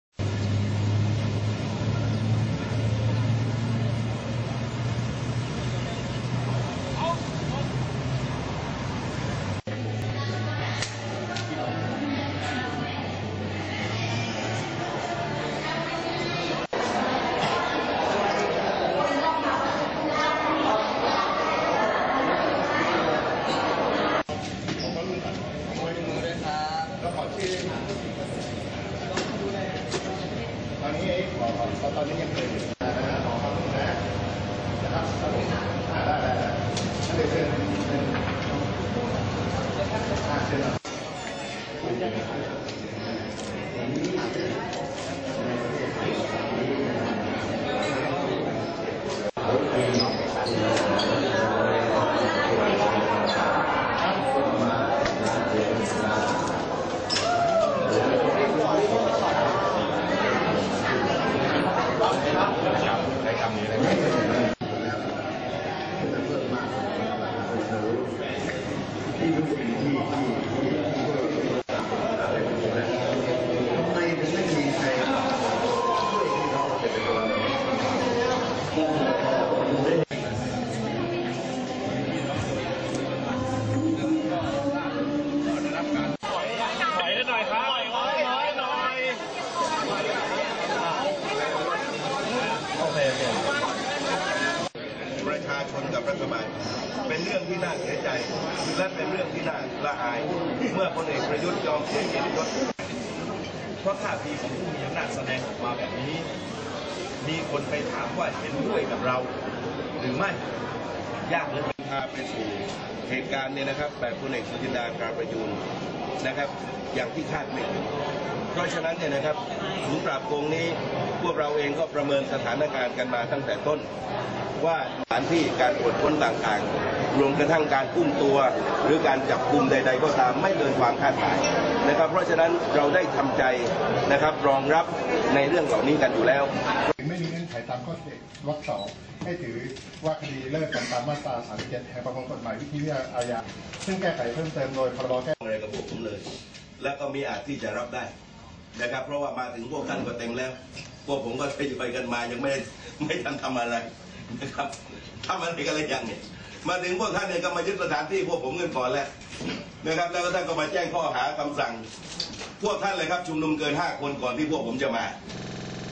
ศูนย์ปราบโกง
เจ้าหน้าที่ตำรวจเข้าควบคุมศูนย์ปราบโกงที่บริเวณชั้น 5 ศูนย์การค้าอิมพีเรียล ลาดพร้าว โดยเข้าเจรจากับแกนนำเพื่อทำความเข้าใจว่าต้องไม่ระเมิดคำสั่ง คสช.โดยใช้วิธีเจรจาพูดคุยทำความเข้าใจกัน โดยไม่มีอะไรกระทบกระทั่งกันหรือเผชิญหน้ากัน โดยทางศูนย์ได้เปลี่ยนป้ายเป็น ประกาศ ที่นี่ไม่ใช่ ศูนย์ปราบโกง ประชามติ

ภาพโดยกุลพันธ์ ศิริพิมพ์อัมพร (Kunlaphun Sirimamporn)